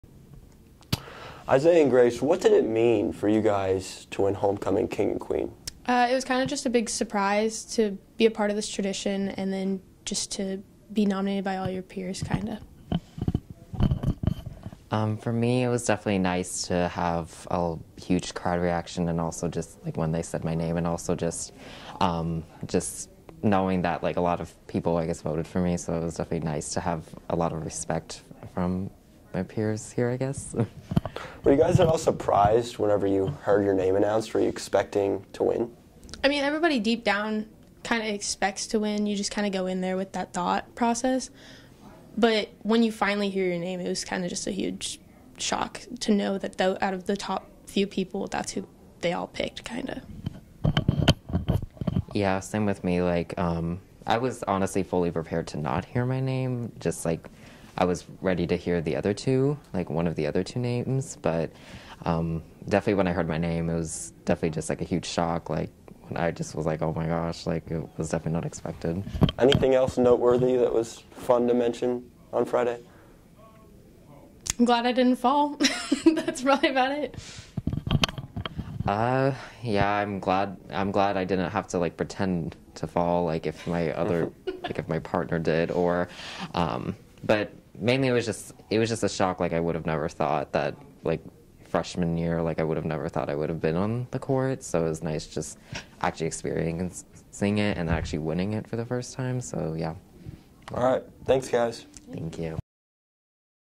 Basketball Homecoming 2022 - Interview
Leo sits down with the 2022 Basketball Homecoming King and Queen.